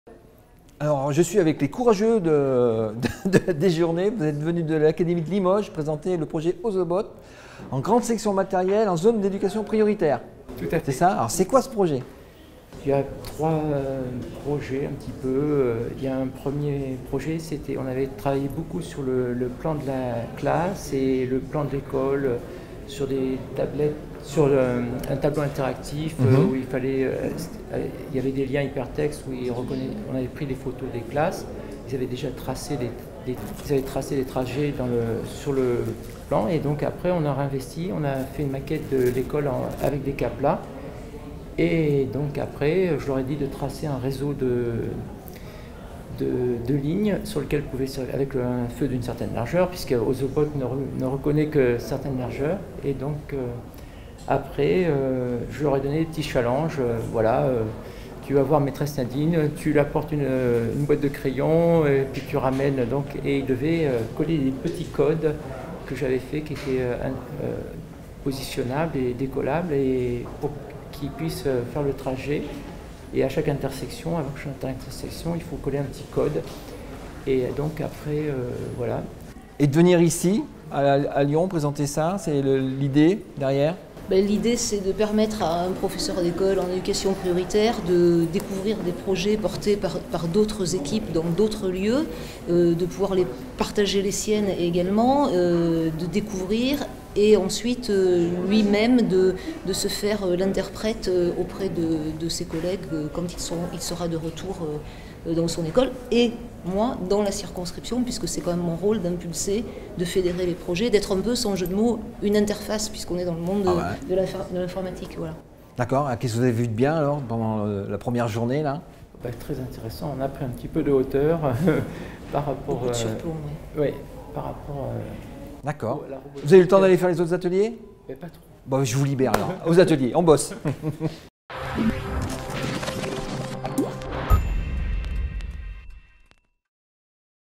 Robot éducation 2016 - Pierre Mialet
Christophe Batier s’entretient avec les participants aux journées robot éducation 2016.